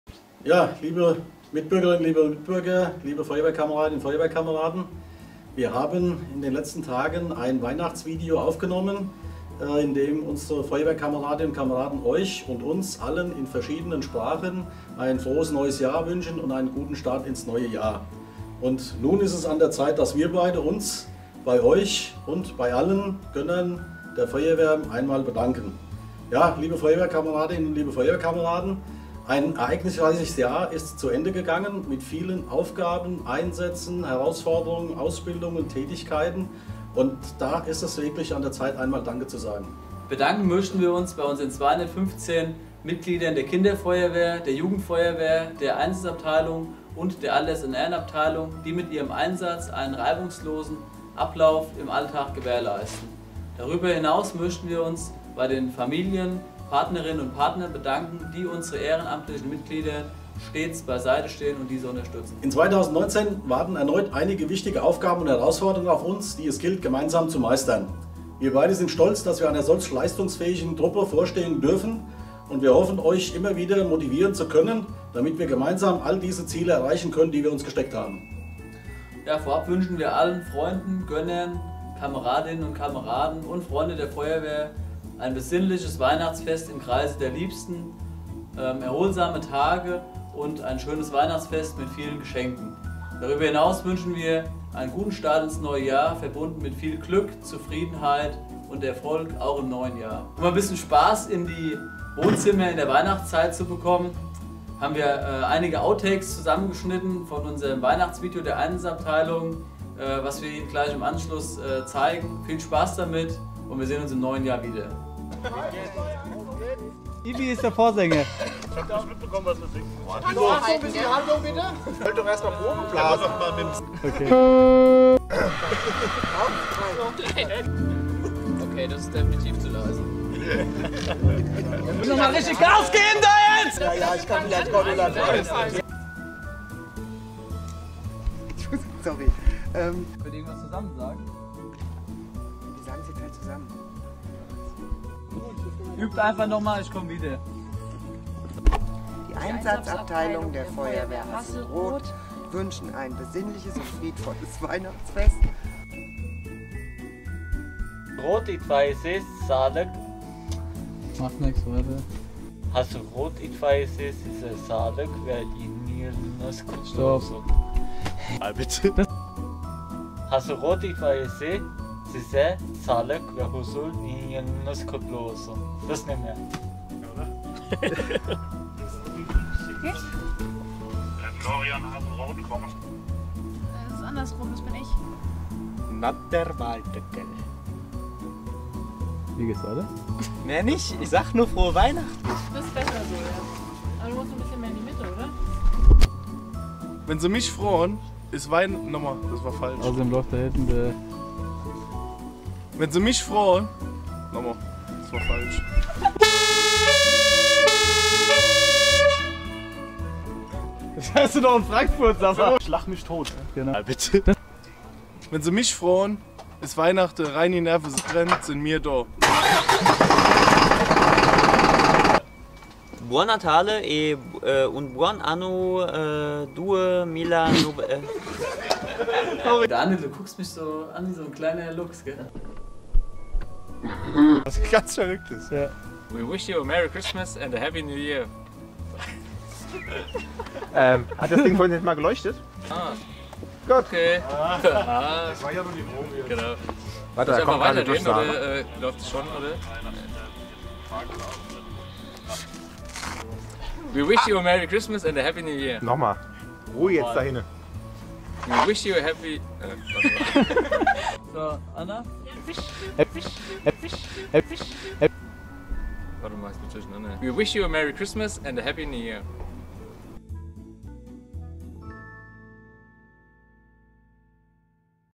Weihnachtsgruß der Gemeindebrandinspektion & Outtakes vom Weihnachtsvideo
Weihnachtsgruß der Gemeindebrandinspektion. Vielen Dank an alle Einsatzkräfte für ihr Engagement, sowie Outtakes vom Weihnachtsvideo der Einsatzabteilung